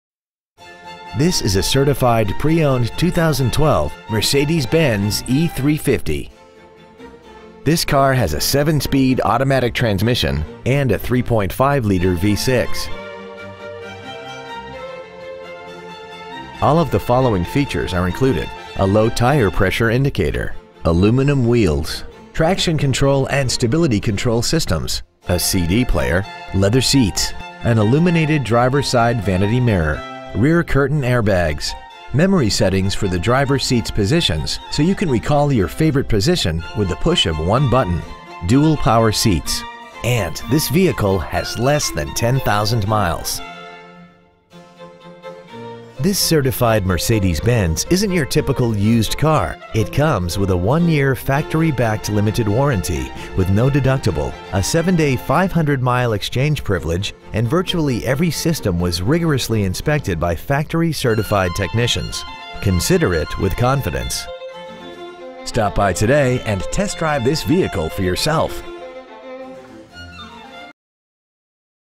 Pre-Owned 2012 Mercedes-Benz E350 Houston TX 77027
We have been honored to serve the Houston TX area , we promise that your experience at our dealership will exceed your expectations ! Year : 2012 Make : Mercedes-Benz Model : E350 Engine : Gas V6 3.5L/213 Trans . : Automatic Exterior : Black Miles : 9,221 Interior : Beige Mercedes-Benz of Houston Greenway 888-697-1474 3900 Southwest Freeway Houston , TX 77027